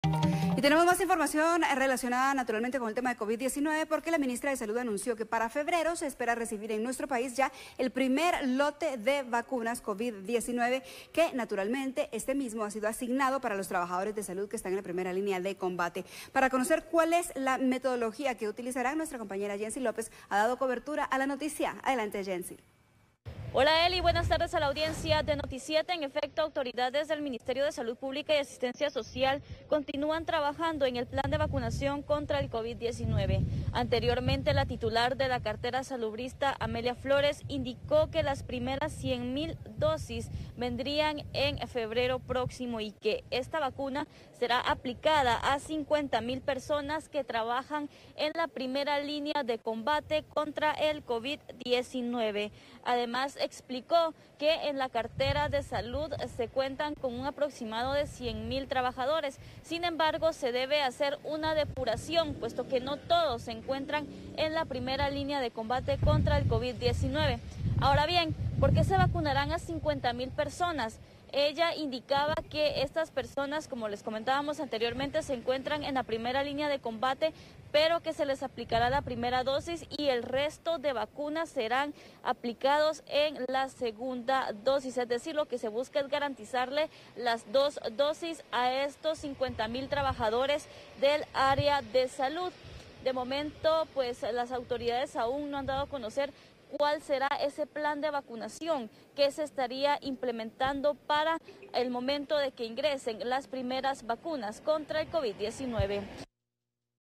¿Cuándo llegarán las vacunas Covid-19 a Guatemala?
La ministra de Salud Pública y Asistencia Social, Amelia Flores, señaló que el primer lote de vacunas contra el Covid-19 arribará durante el mes de febrero.